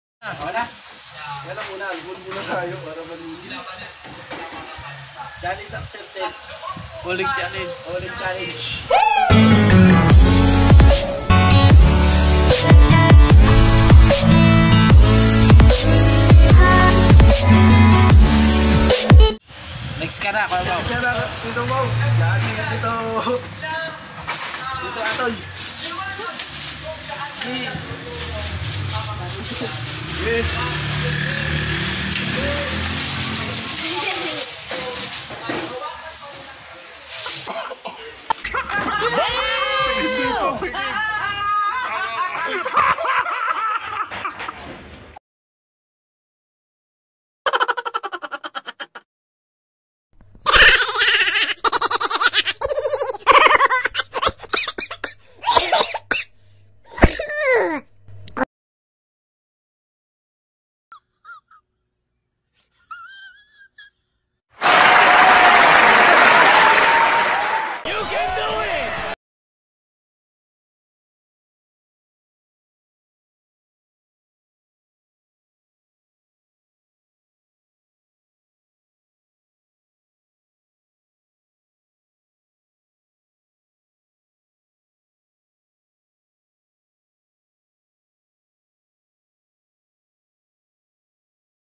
Funny videos Uling Challenge
Uling Challenge iba talaga ang pinoy.
pamangkin kong game na game sa challenge.
it's more fun in the Philippines.